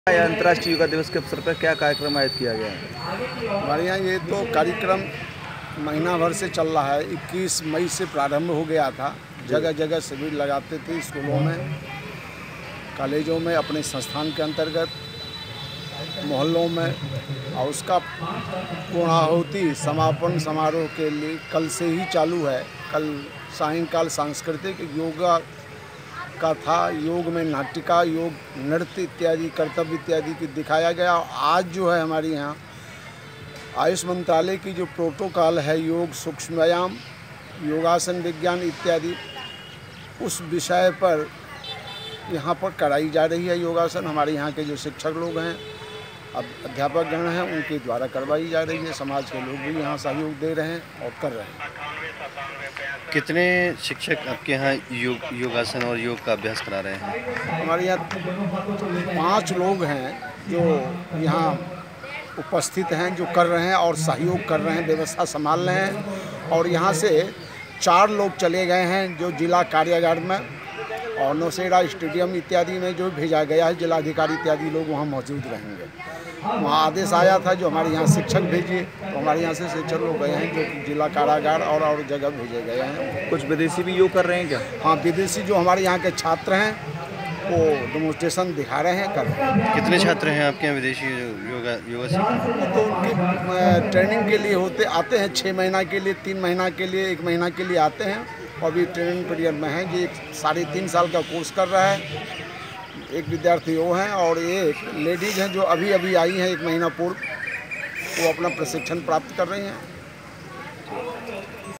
मथुरा योग दिवस
वृन्दावन, अंतरराष्ट्रीय योग दिवस के उपलक्ष्य में श्री संकट मोचन योग अनुसंधान संस्थान के बैनरतले स्थानीय गांधी पार्क परिसर में योगशाला का आयोजन किया गया, जिसमे शिक्षकों द्वारा सहभागिता करने आये सैकड़ो देशी बिदेशी भक्तों एबं स्थानीय लोगों को बारिकी सेयोग के गुर सीखाये गए,
इस

इस संबंध में जानकारी देते हुए संस्थान निर्देशक धर्मनाथ शास्त्री ने बताया कि संस्थान द्वारा एक माह पूर्ब ही लोगो को योग क्रिया सिखाने के लिए कई स्थानों पर शिविरो का शुभारंभ कर दिया गया था, जिसका समापन योग दिवस की पूर्ब संध्या से ही शुरू हो गया जिसमें साय काल सांस्कृतिक कार्यक्रमो के माध्यम से योग नृत्य, योग नाट्य, योग कला आदि की प्रस्तुति दी गयी वही उन्होंने बताया कि गुरुबार को योग दिवस के उपलक्ष्य में प्रातः काल आयुष मंत्रालय की प्रोटोकॉल योग सुक्षायाम, योगासन विज्ञान इत्यादि के बिषय पर लोगो को योगाशन कराया गया, कहा कि आज युग मे योग ही एक साधन है जिससे निरोगी रहकर अपना जीवन सुख के साथ ब्यतीत किया जा सकता है, इस दौरान जहा नगर की राजनीतिक हस्तियां मौजूद रही वही सेकड़ो की संख्या में उपस्थित देशी विदेशी भक्तों एबं युबाओ ने भी बारिकी से योग के गुर सीख निरोगी रहने का संकल्प लिया।